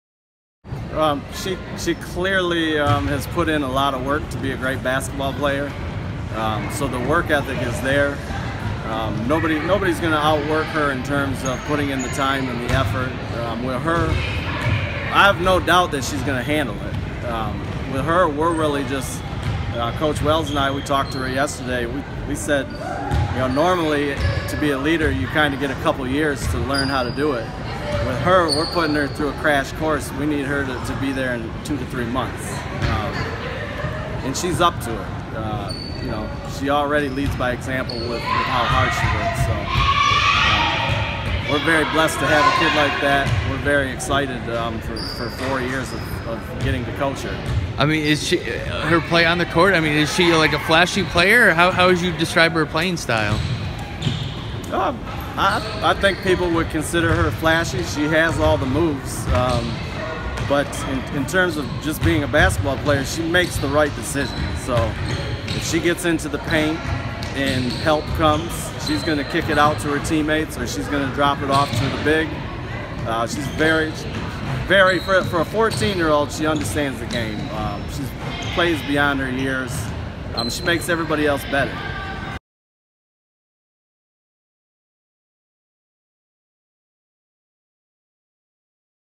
Arbor Prep coach raves about freshman phenom
Arbor Prep coach Scott Stine on freshman Mya Petticord, who already has seven DI offers.